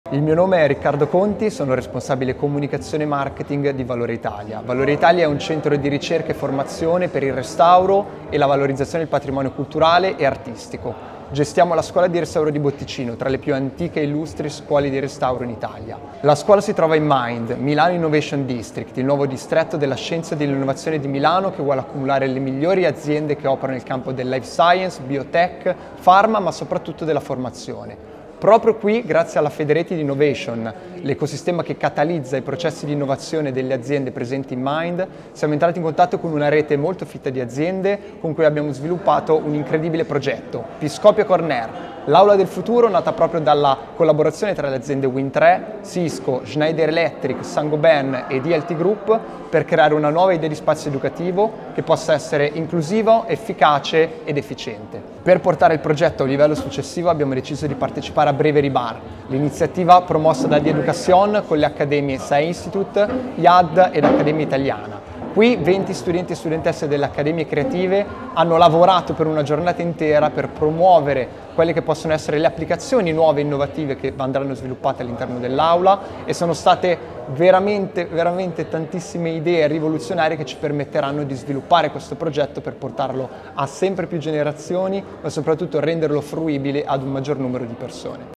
Abbiate “fame” di curiosità - Riccardo Conti⎪IAAD.
“Abbiate “fame” di curiosità, che non sia solo sui libri, ma che si espande nella vita quotidiana.”
Questo il prezioso consiglio di Riccardo Conti, responsabile della Comunicazione e Marketing di Valore Italia.

Gli studenti e le studentesse di IAAD. Accademia Italiana e Sae institute hanno immaginato l’aula del futuro: un luogo pensato per le comunità e aperto a stimoli molteplici a un pubblico diversificato, per rendere la formazione accessibile a tutti e a tutte.
Questo  workshop realizzato in collaborazione con Windtre, Schneider Electric, Saint-Gobain Italia, ELT Group VALORE ITALIA Impresa Sociale nel contesto di Federated Innovation @MIND, in occasione del BRA-VERY BAR 2024!